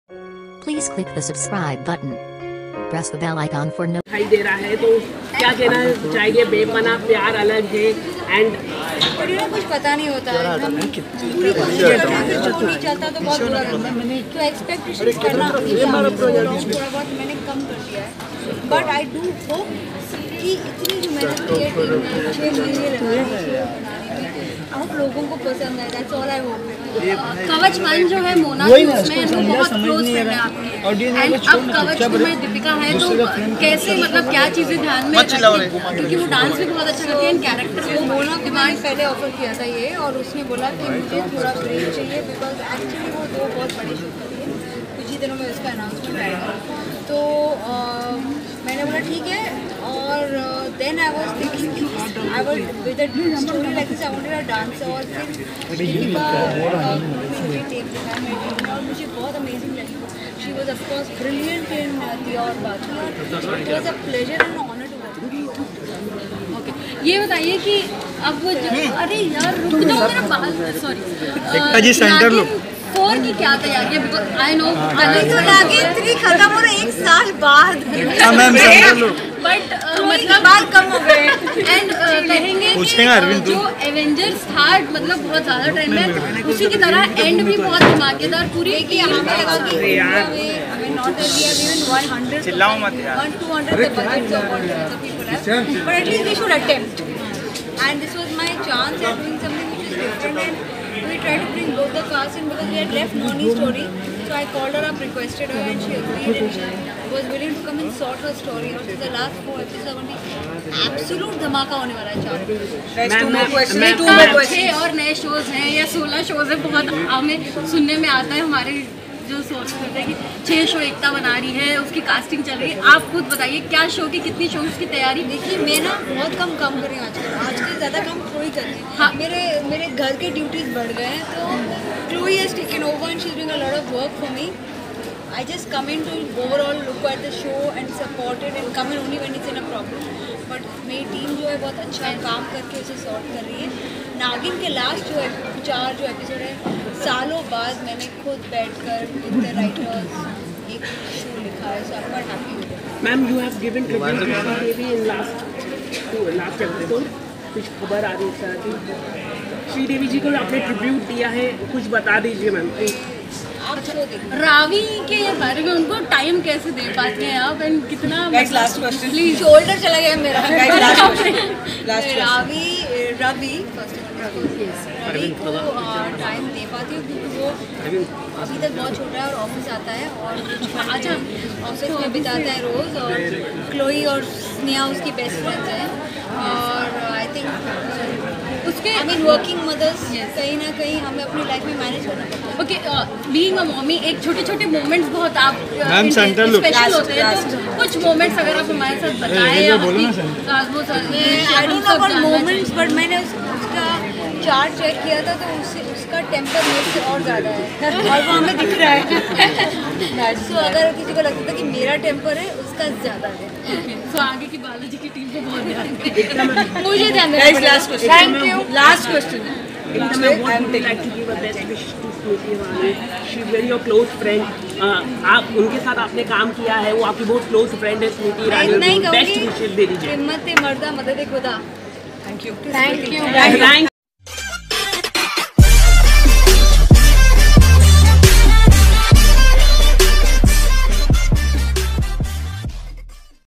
Ekta Kapoor Talk About Her Colors New Show Kawach 2 & Bepanah Pyaar
Ekta Kapoor Talk Colors New Show Kawach 2 & Bepanah  Pyaarr